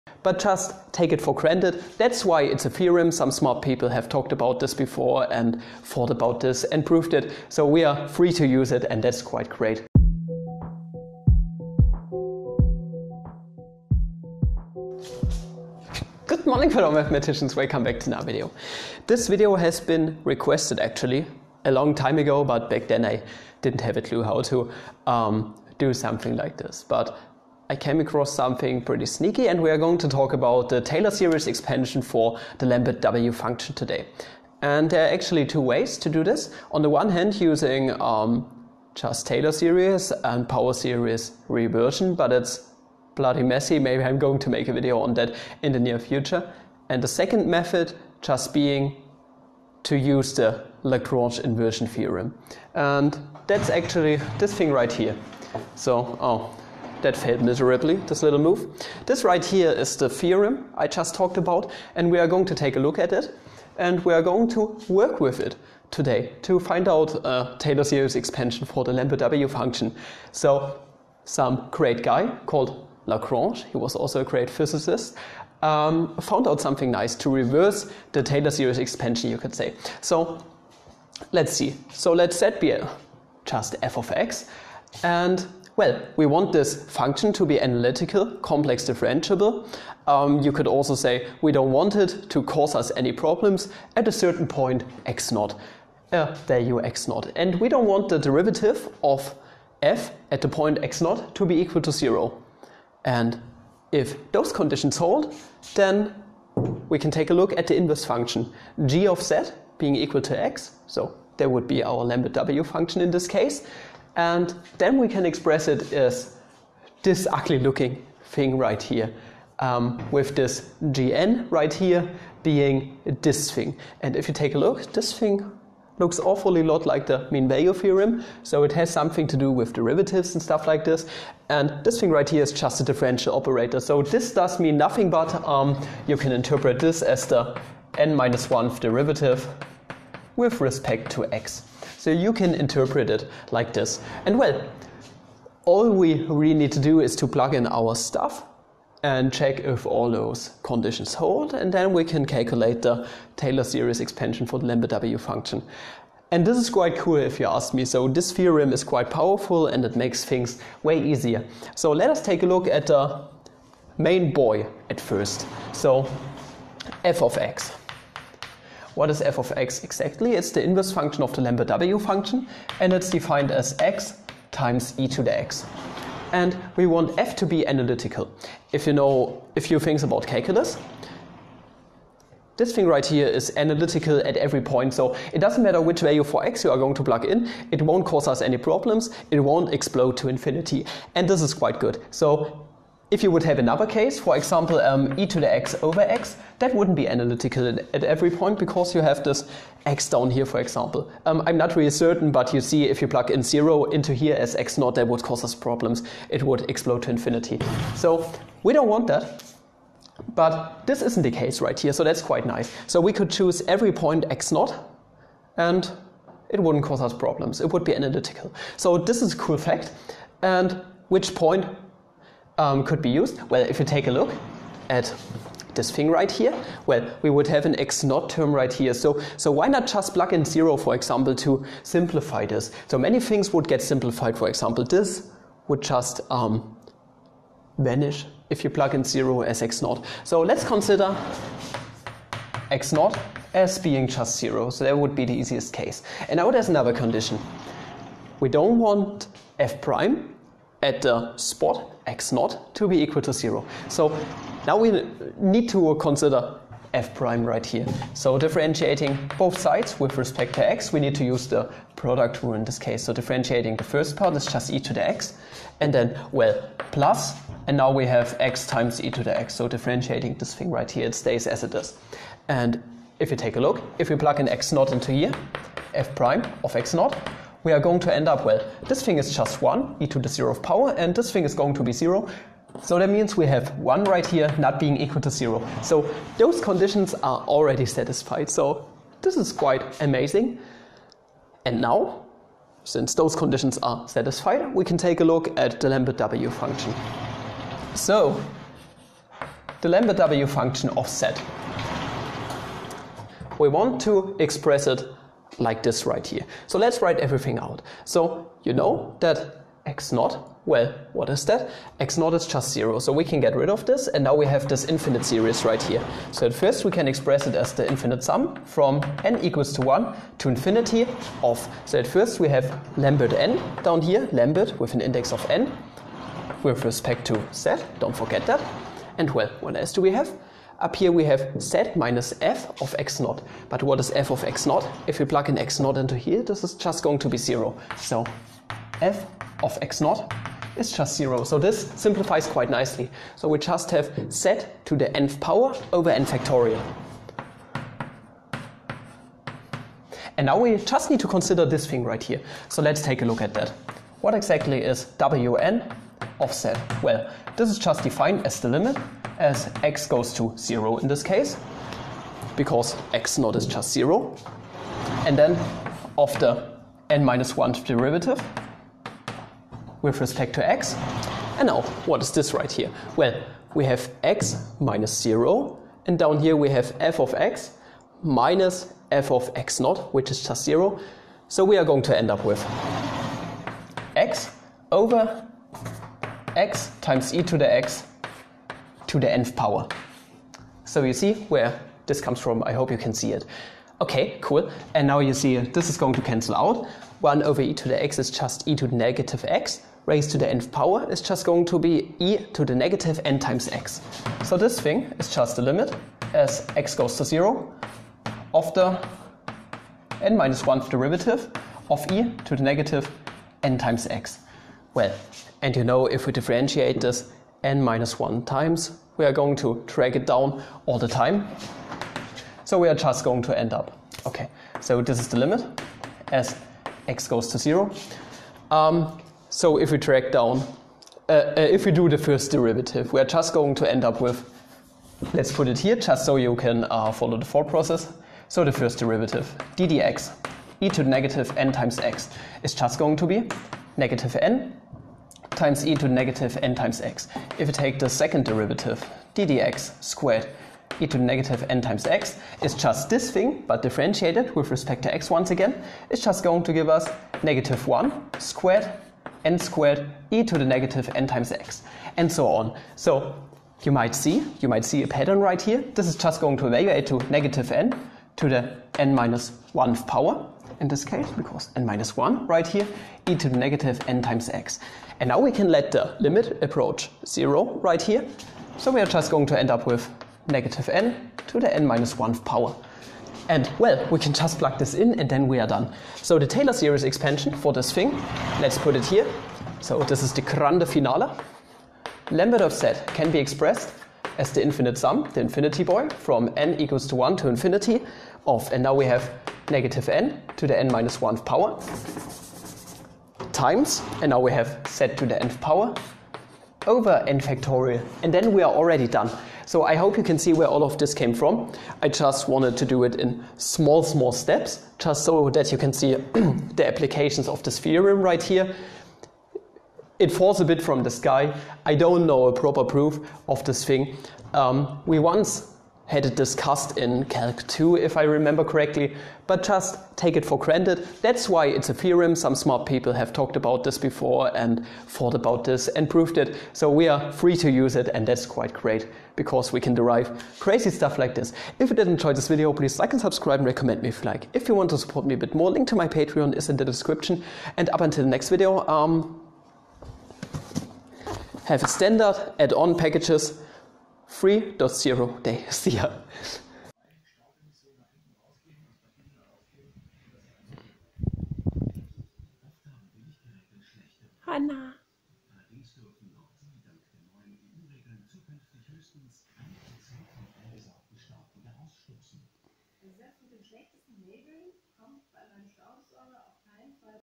Lambert W Taylor Series Expansion [ Lagrange Inversion Theorem ]
Let us do something special today! =) We are going to use the agrange Inversion Theorem to find out a Power Series Representation for the Lambert W Function! I hope you are going to enjoy this treat! :)